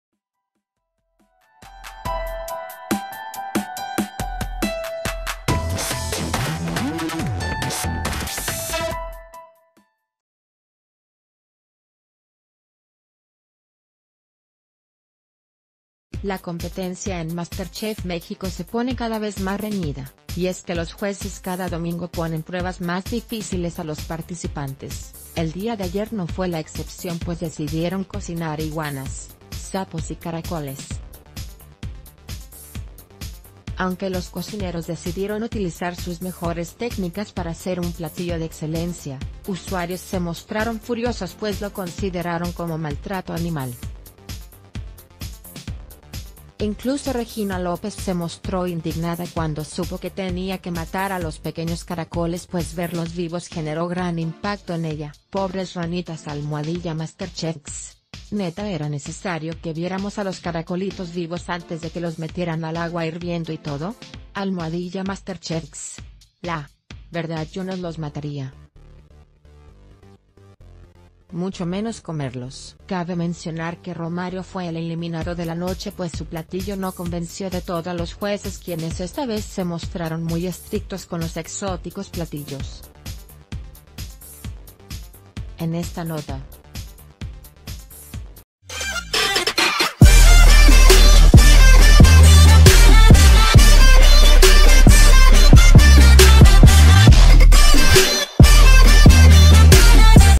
Fans de MasterChef México furiosos por cocinar animales vivos
La competencia en MasterChef México se pone cada vez más reñida y es que los jueces cada domingo ponen pruebas más difíciles a los participantes, el día de ayer no fue la excepción pues decidieron cocinar iguanas, sapos y caracoles. Aunque los cocineros decidieron utilizar sus mejores técnicas para ...